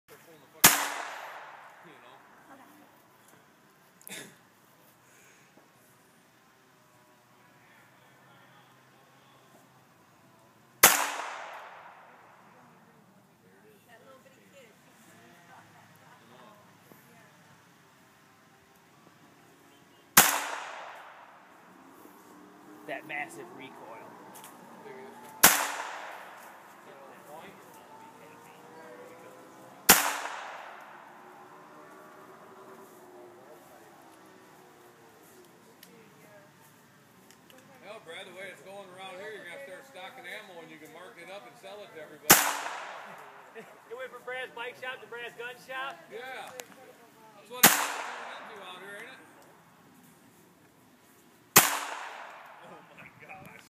Dawson shoots pistol
6 yr old dawson shoots .22 pistol